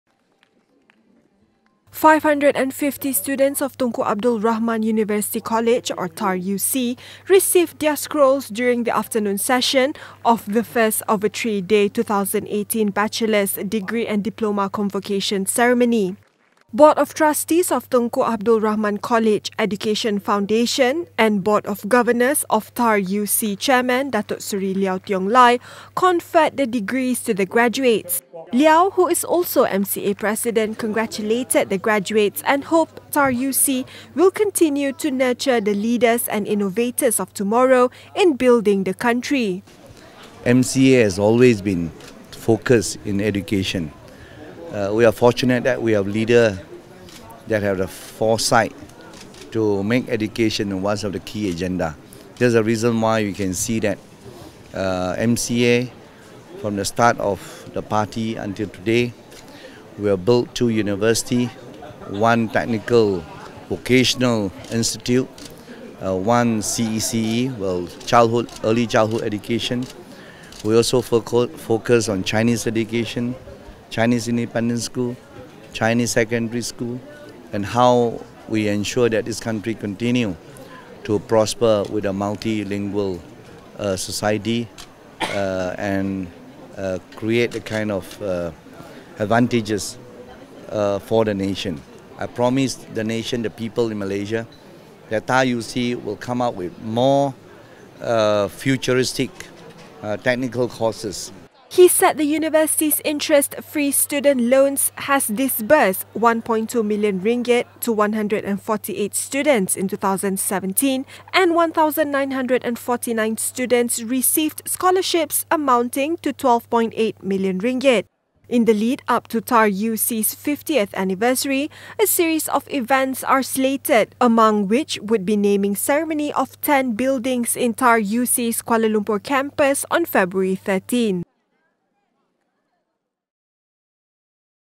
Liow: TAR UC will continue to produce high-calibre graduates
550 students of Tunku Abdul Rahman University College or TAR UC received their scrolls from Datuk Seri Liow Tiong Lai during the afternoon session of the first of a three day 2018 Bachelor’s degree and Diploma convocation ceremony.